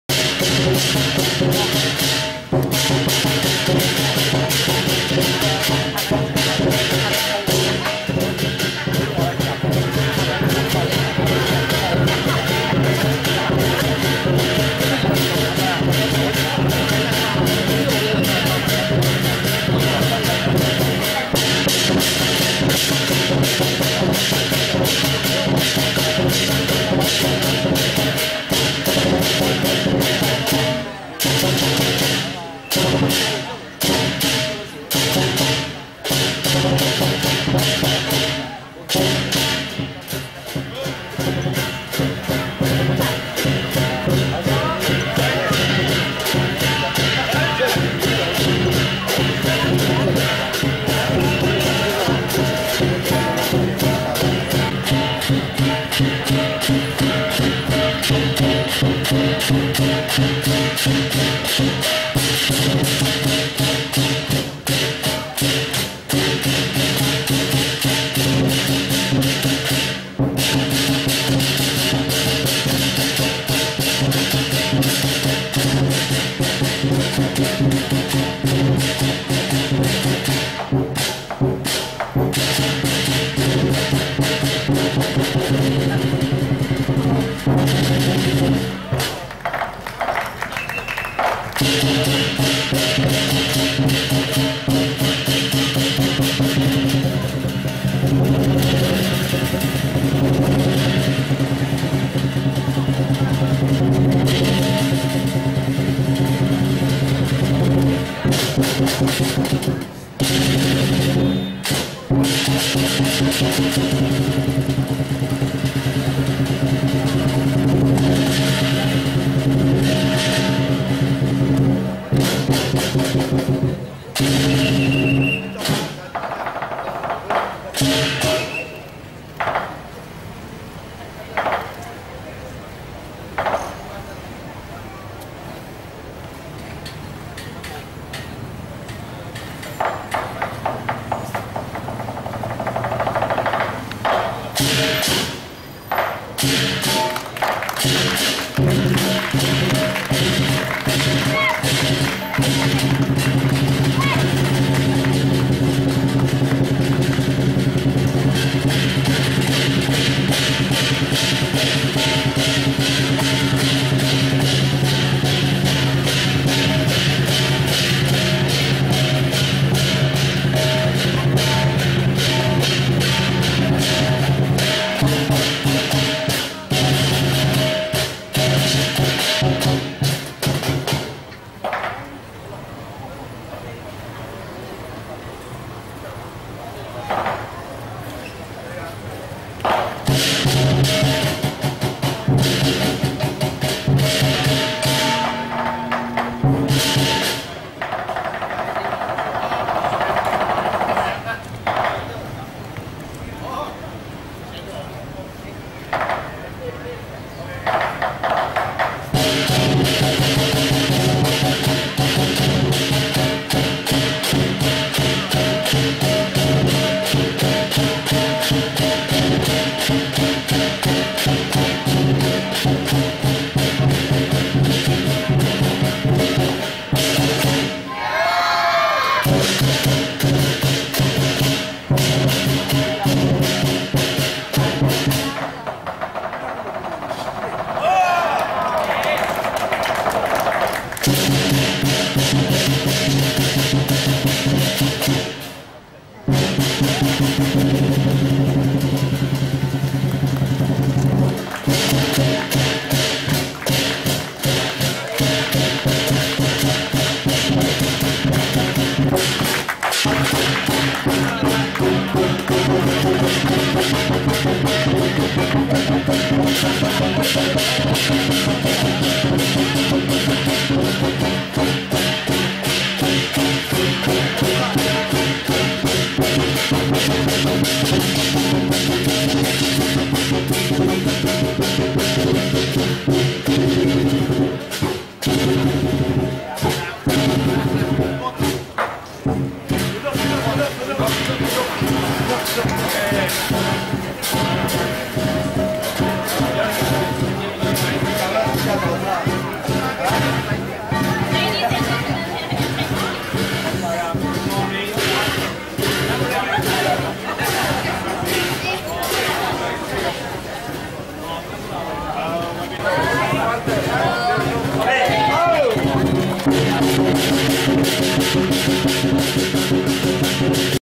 OPENING OF SOLAR POWERED TAXI STAND KG AIR KOTA KINABALU 16FEB2015
This taxi stand has a modern solar cell complete with storage batteries and control system to power just the lights and fans, in Kota kinabalu

So the donor had a glamour opening ceremony as shown